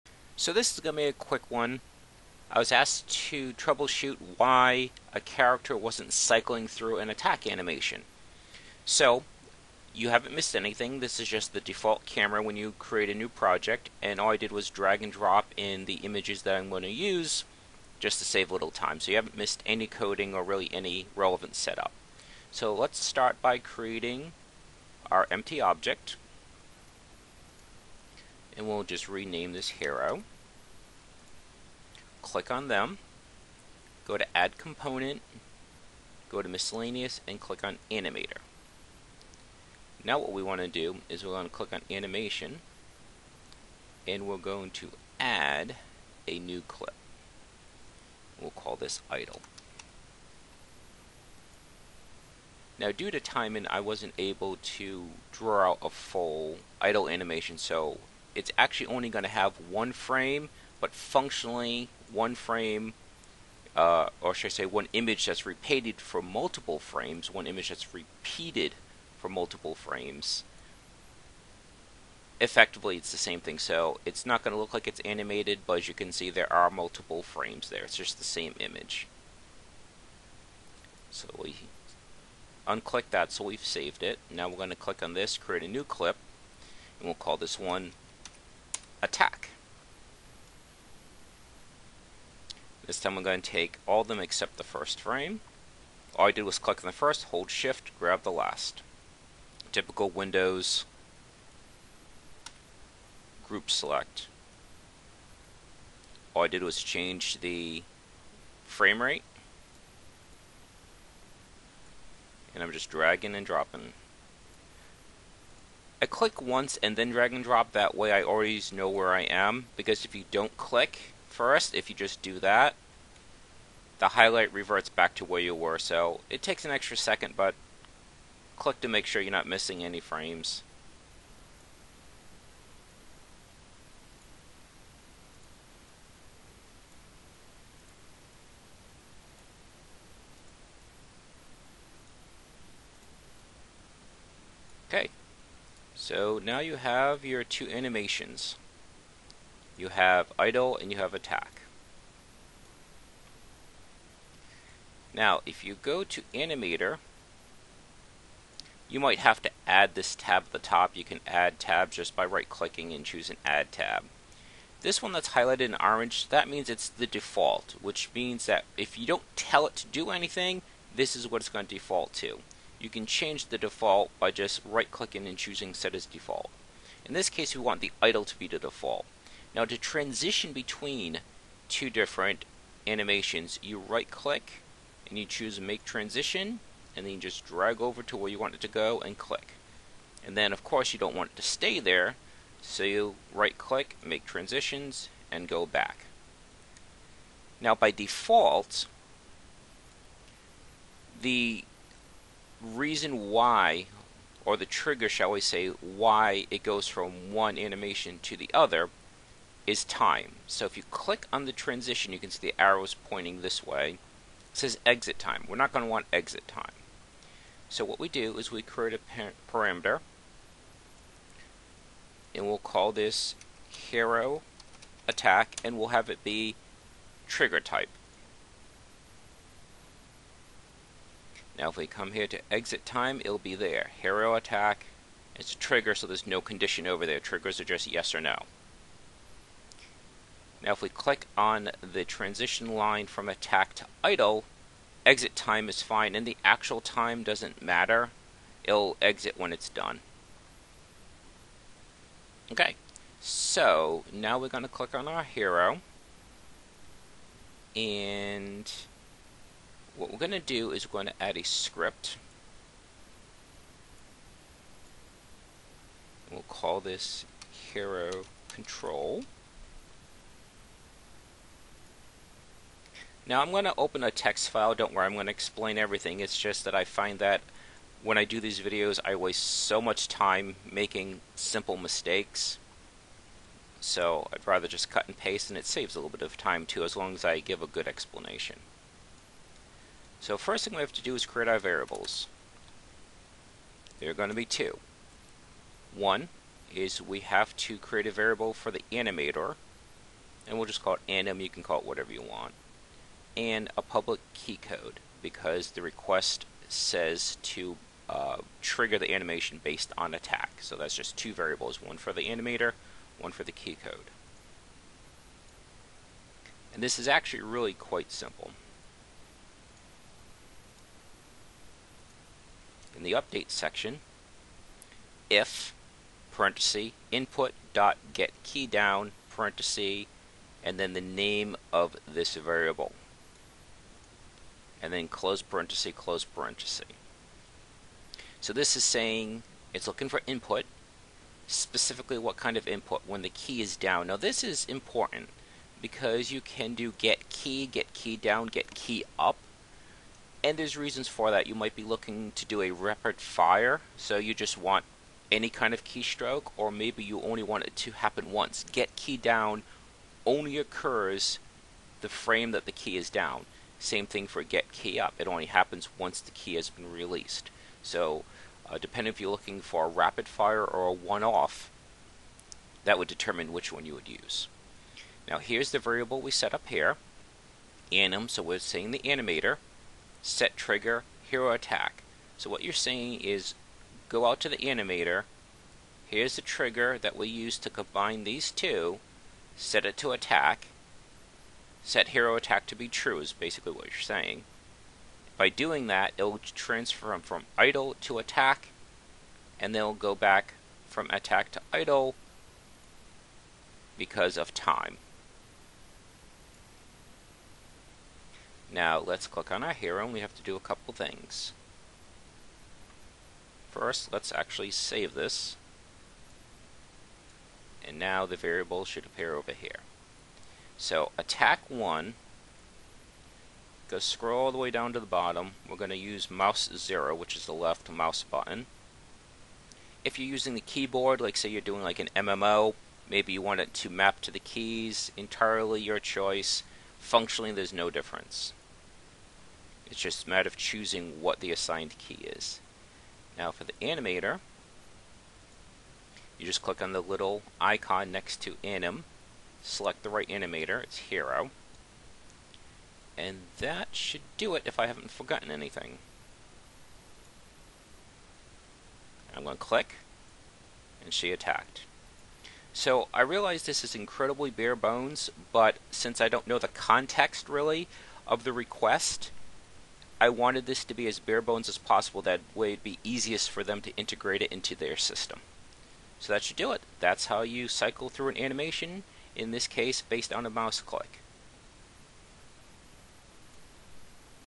Unity C# - Change Character Animation with Mouse Click
By request, here is a short video that shows how to create player animations and switch between them with a key press.